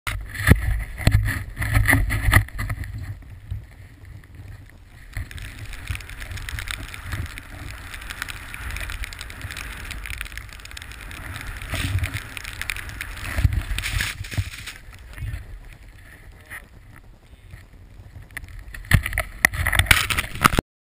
A short clip of Damo riding powder at Seki Onsen
You know when the man is riding with a shovel in hand he knows digging himself out is on the cards.